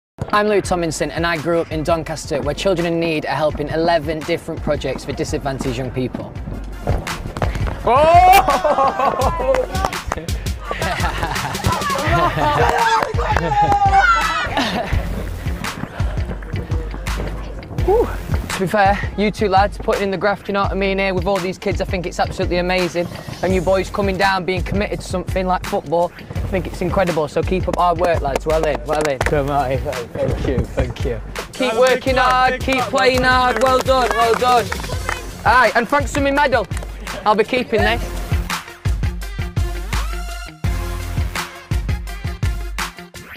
Louis Tomlinson gets his football boots on to visit a project supported by BBC Children in Need
Louis Tomlinson